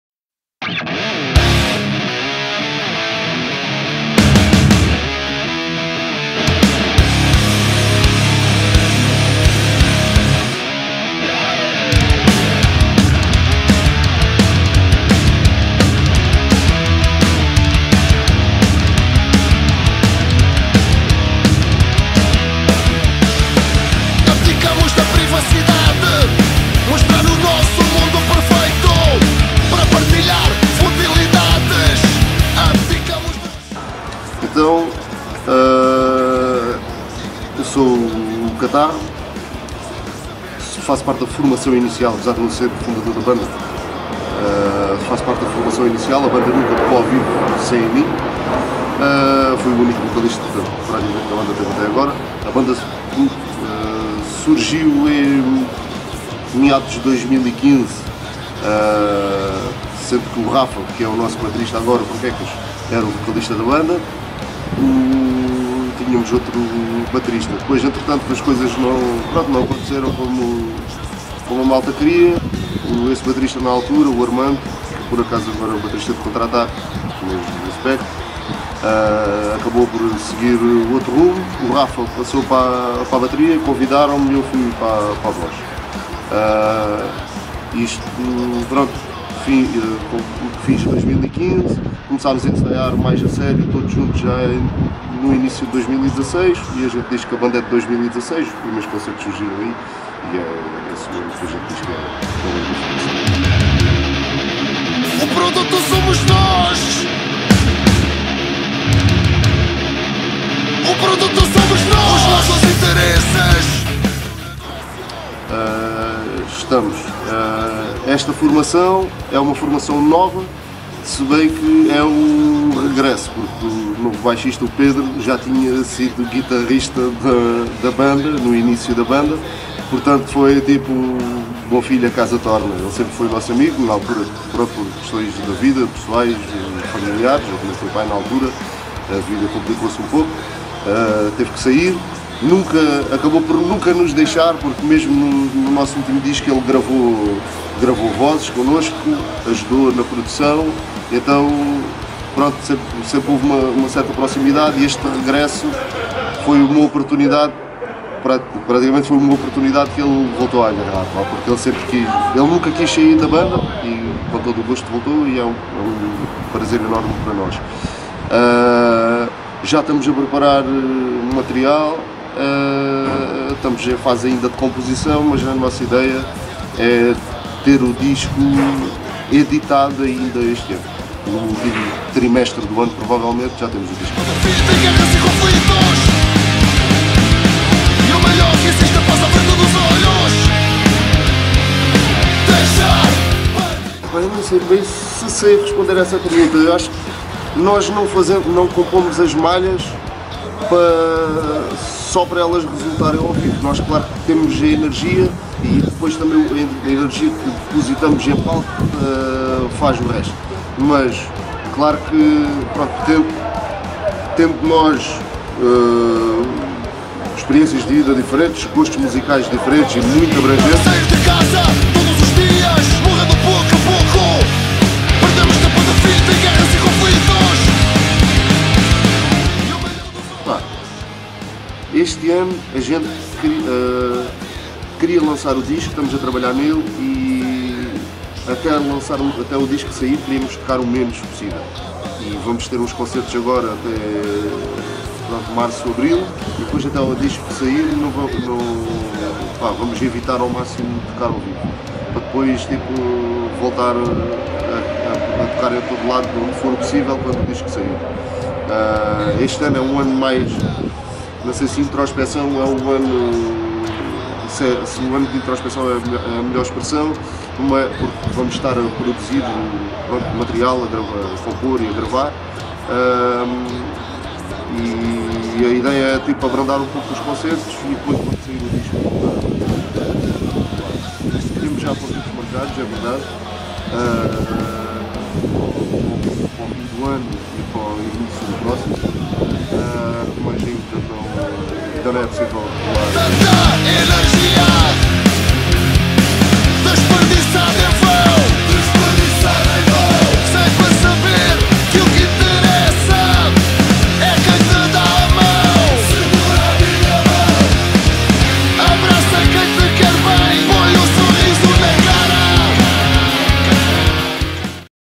HARDCORE LIVES - RICARDO CATARRO / M.E.D.O. by [xsaladxdaysx]
The video recording of the interview was conducted and done by myself @ Alvalade, in Lisbon (Portugal). Thanks to all involved.